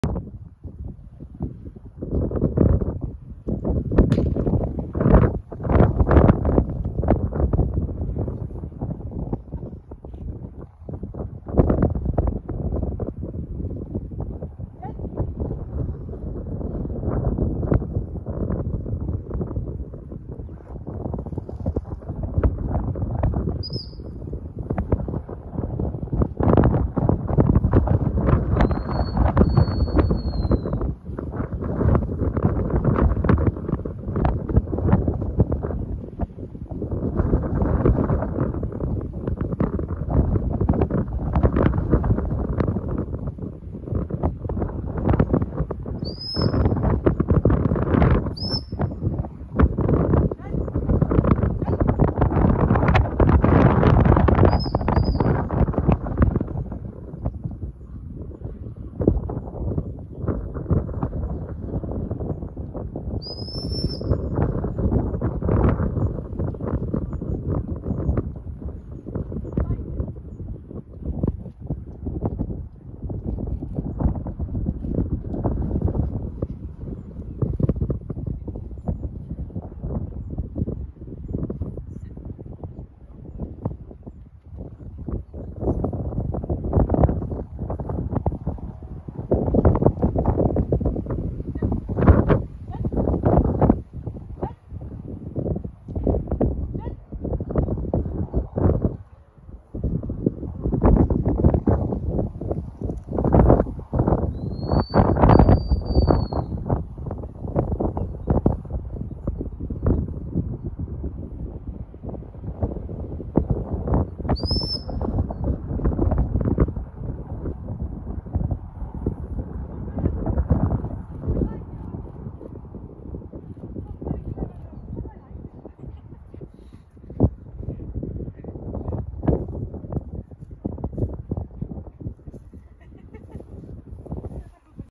WT Milovice - 16.3.2024 - Beginners (L) class, Ares (Adventure Boy Ledové Moře)
Retriever Sport CZ
Rozhodčí: Steve Cullis, Paula Cullis (UK)
Výcvik Retriever s Romanou